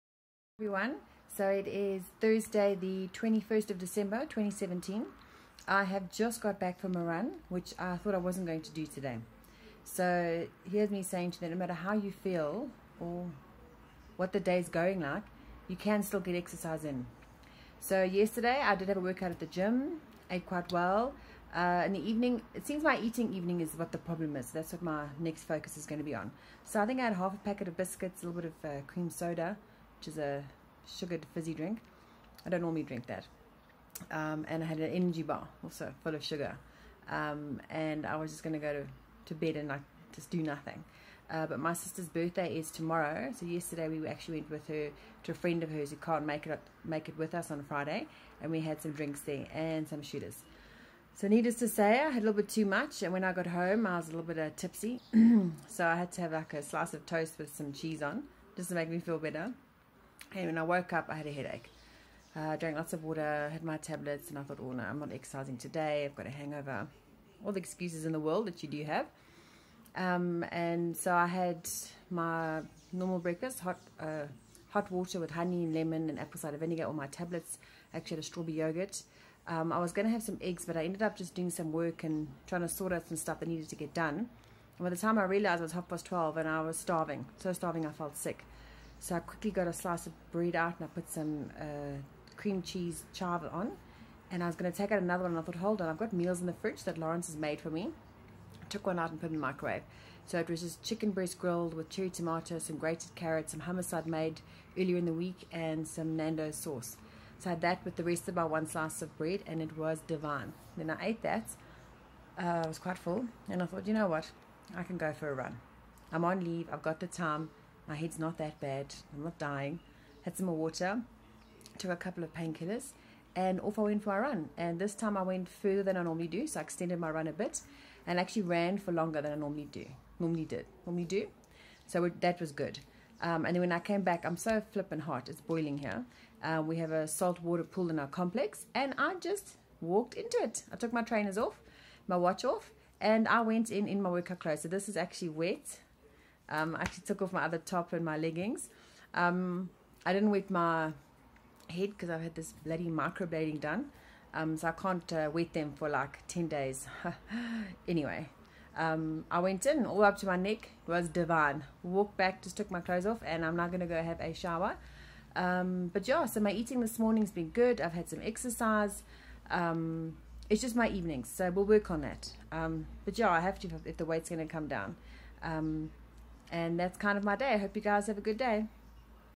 Imo's Health and fitness update - Thursday 21st December 2017
Here's me telling you, you can exercise no matter how you originally feel.
Woke up with a slight hangover and assured myself no workout was happening today. Well I went for a run after lunch and feel fabulous!!